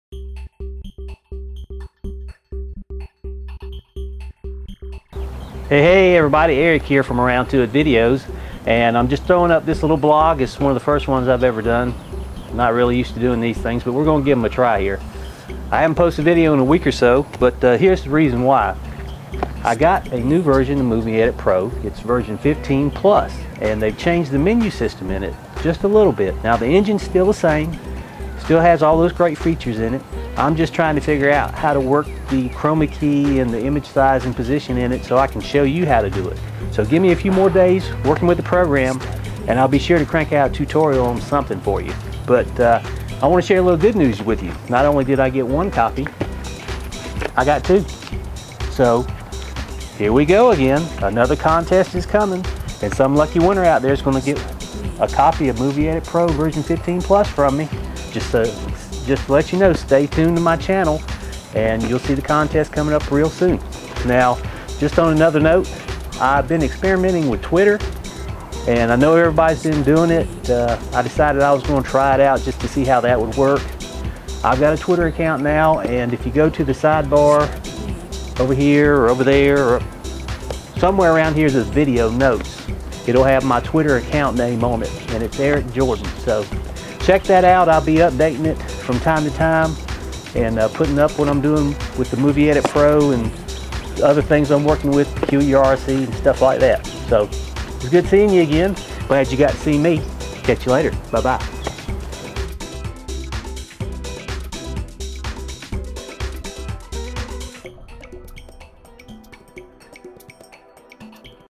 Movie Edit Pro V15+ Contest and Twitter Vlog
Here is the link to get Movie Edit Pro.

Just a quick shout out to everyone to let you know I am still here and experimenting with the latest version of Movie Edit Pro V15+.

Also, I am on twitter now and will be leaving updates there as to whats going on at Around Tuit Videos.

Here is a link to my twitter page.

Thanks,

Eric